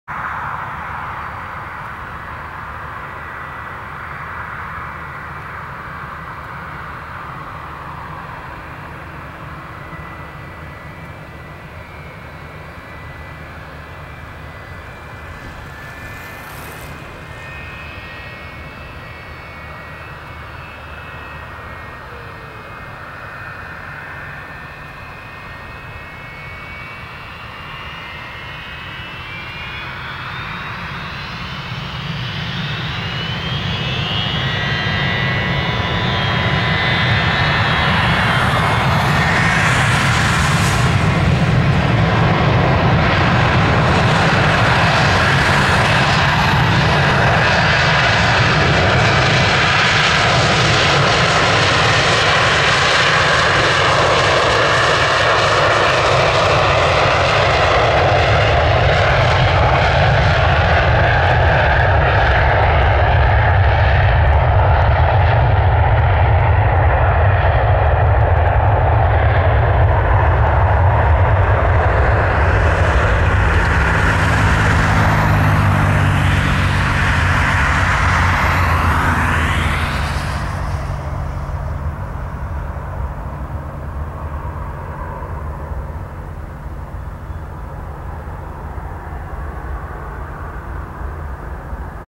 American Airlines N962TW MD83 Takeoff Portland Airport (PDX)
Flight: AAL1220 PDX-DFW